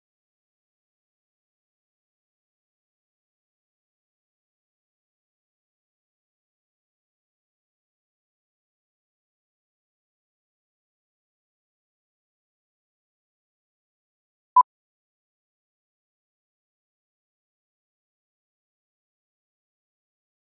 Making Coffee 03 / Free Stock Footage
Description:
Close-up shot of putting sugar into a cup of coffee and stirring it with a spoon.